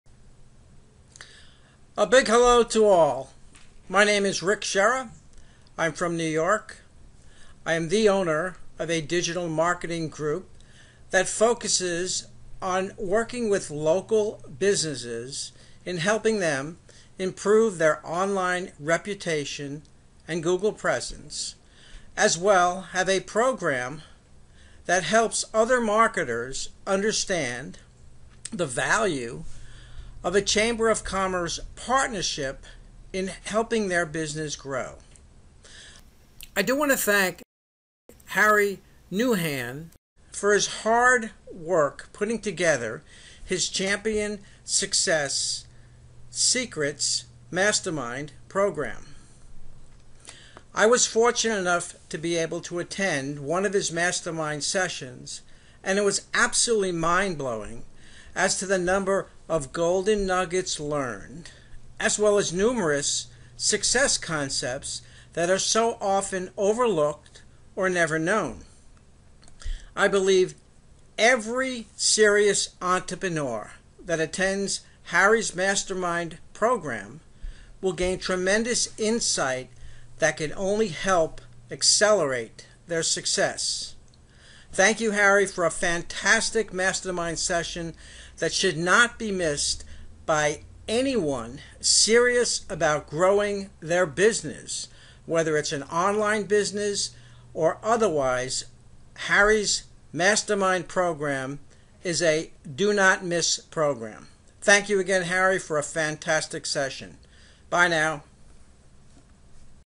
Harry Nouhan Mastermind Program
Personal Review of Harry Nouhan's Saturday's Mastermind Program. Great Session Harry! Not to be missed by anyone serious about growing their business.